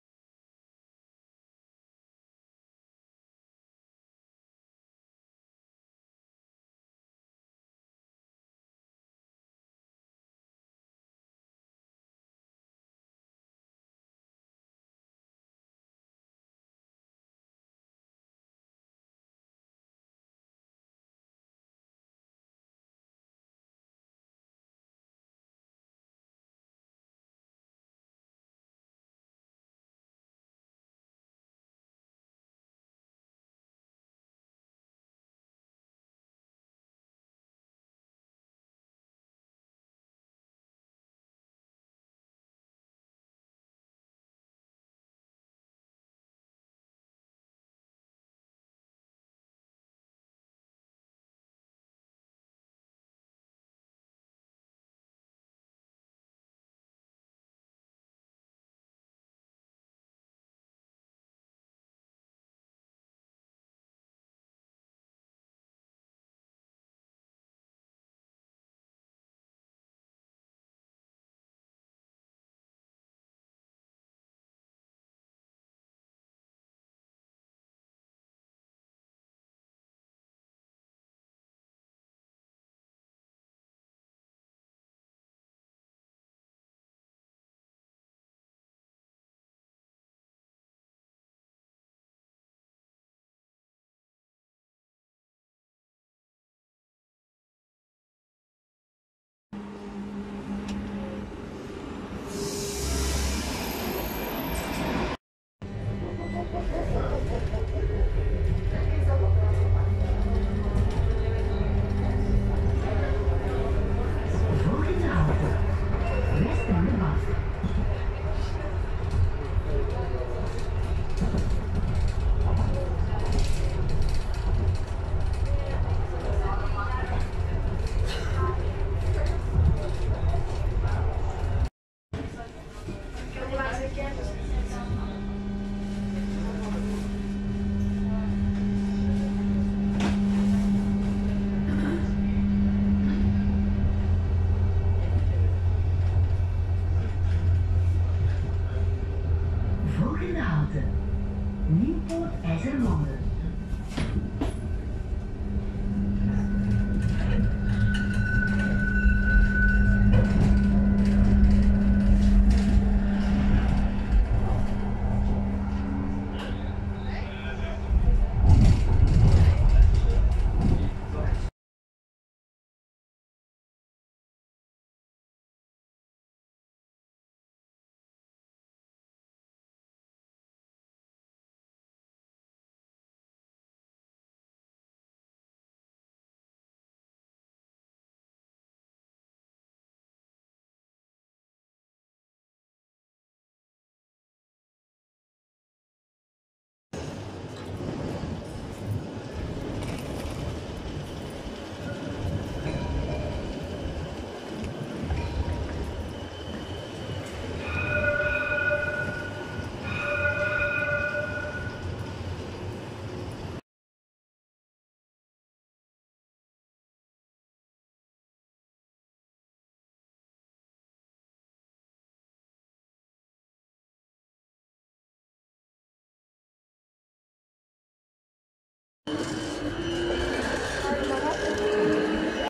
#3 Real Life Trainspotting Video - Ghent & Biggest tramline!
In this video i went spotting some trams in Ghent, and i also went to the biggest TramLine in the world!